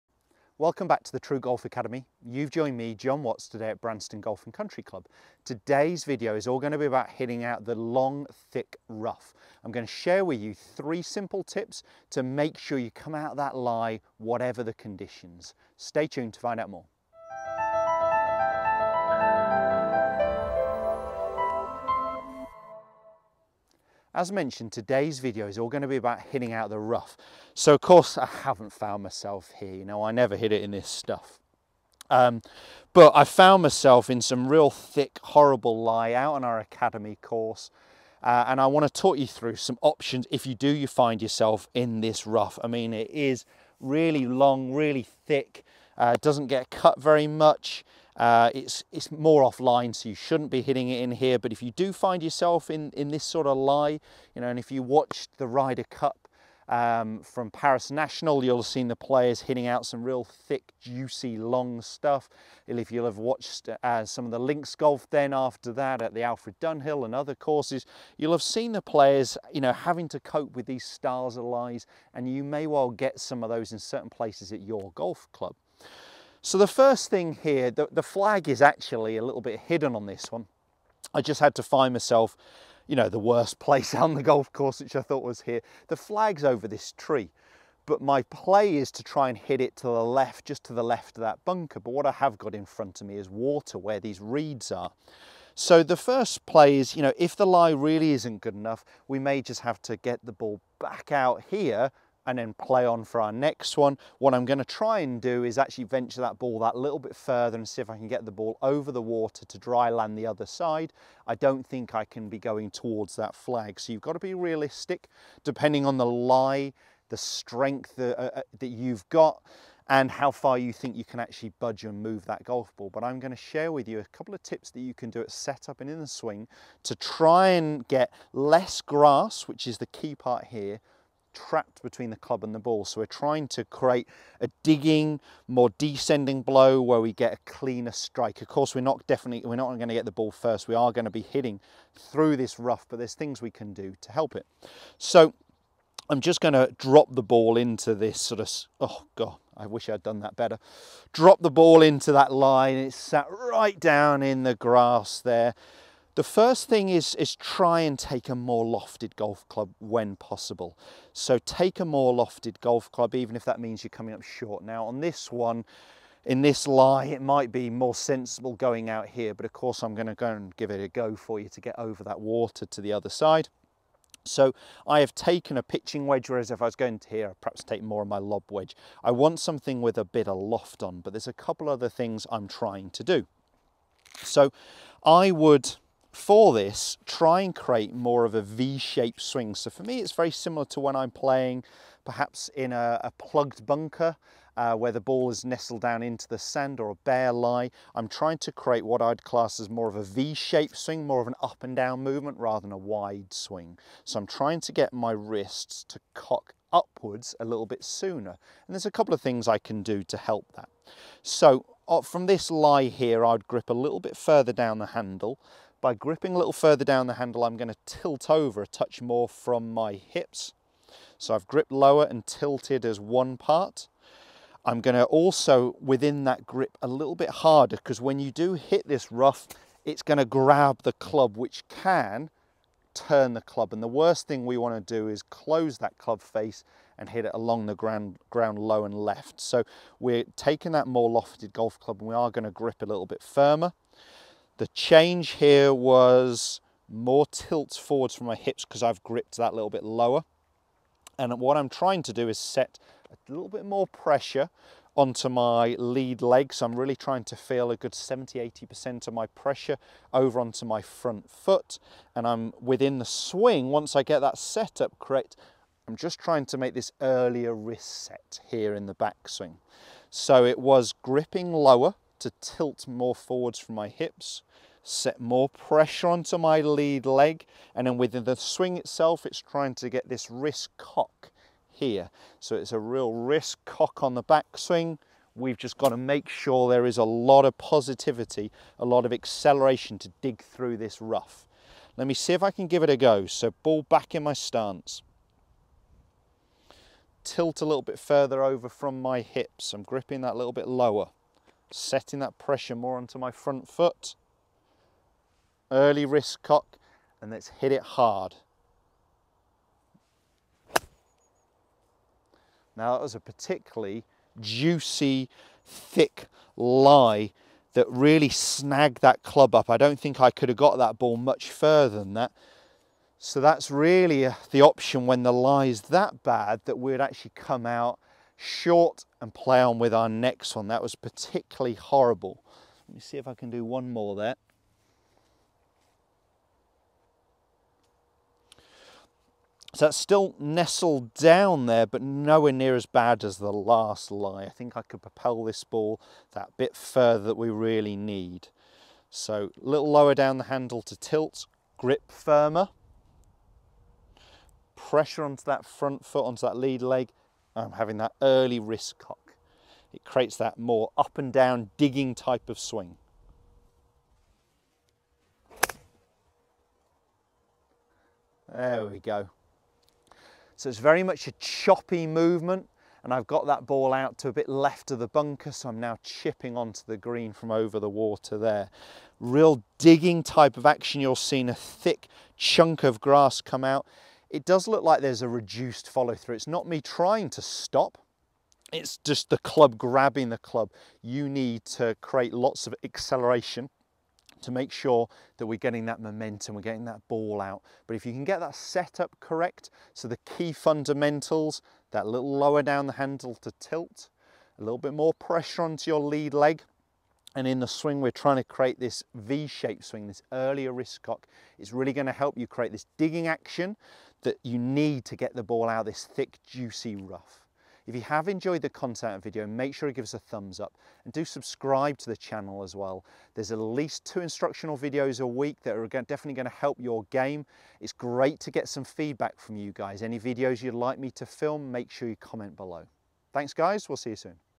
3 SIMPLE STEPS TO PLAY OUT OF THICK ROUGH
Director of Instruction at the TruGolf Academy Jon Watts talks you through 3 simple changes you need to make when pitching from thick rough.

TruGolf Academy is based at Branston Golf & Country Club in Staffordshire, UK.